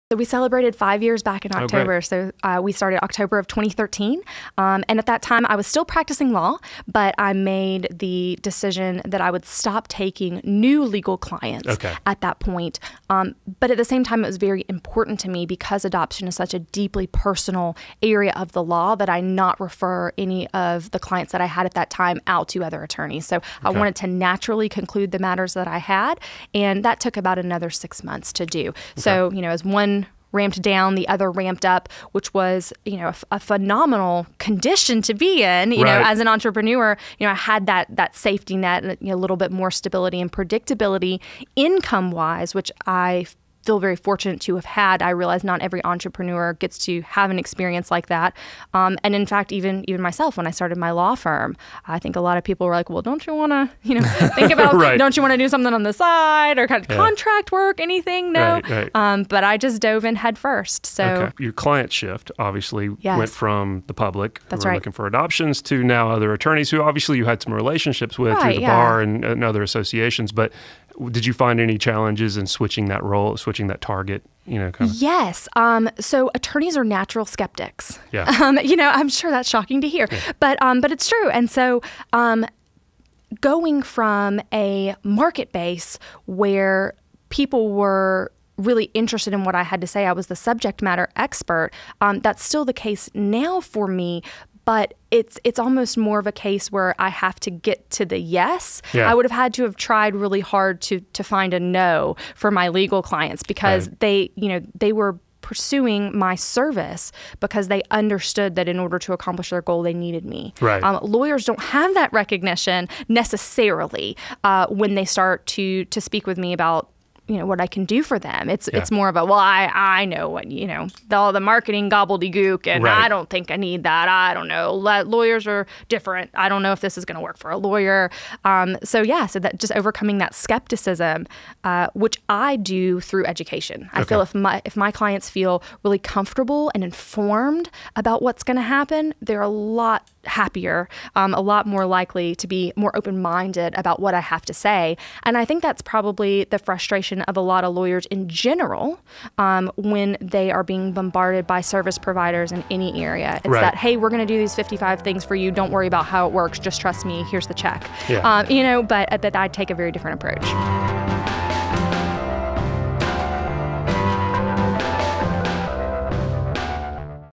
Transition From Law Practice to Legal Marketing Consultant - Heather Riggs - Websuasion Conversation
In the seventh episode of Websuasion Conversation, Heather Riggs of Atlanta Legal Marketing explains the transition from practicing law to providing marketing catered to law practices. She shares how the shift affected her sales process, which she does through educating clients.